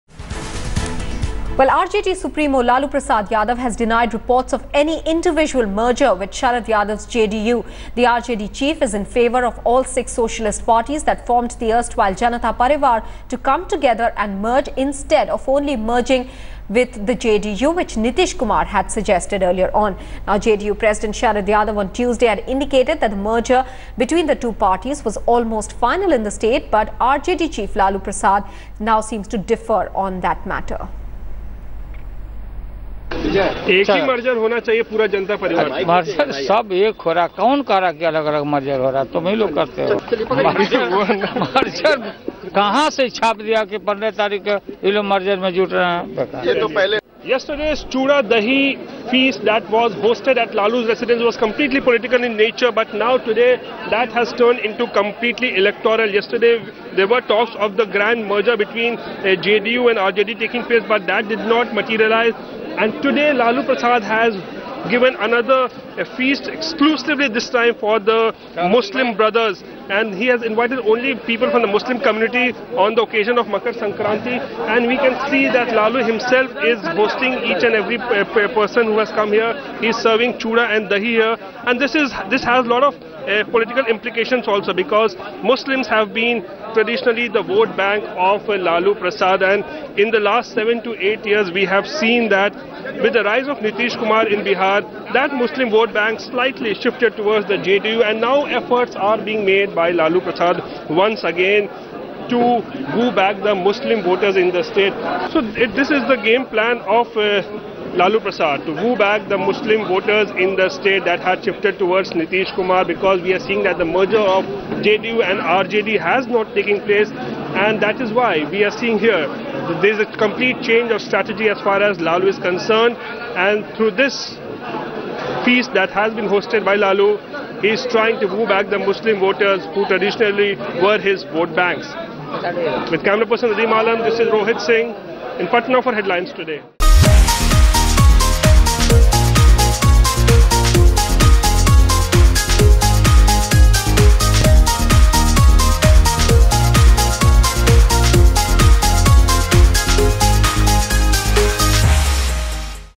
Lalu's chooda-dahi feasts have political, electoral agendas
Yesterday's chooda-dahi feast hosted at Lalu Prasad Yadav's residence was political in nature where word got out that there is no merger happening between RJD-JD(U). Today, however the feast seeks to win the Muslim vote bank, inviting people only from the Muslim community.